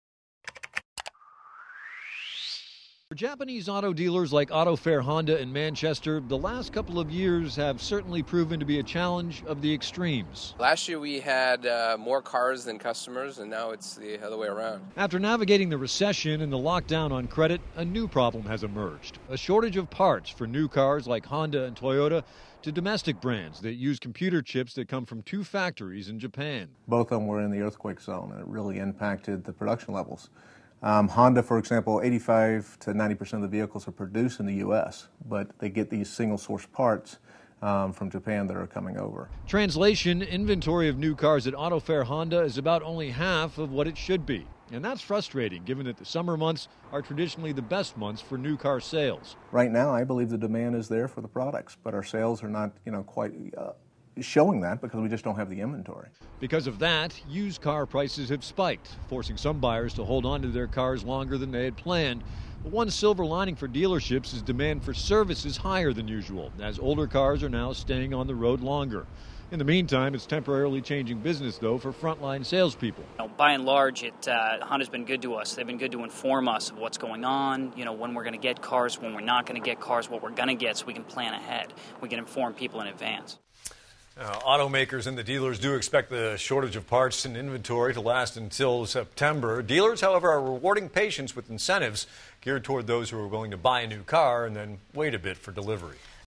Auto Supplies Constrained For Some Dealers
Months after an earthquake and tsunami in Japan, local auto dealers are still feeling the effects.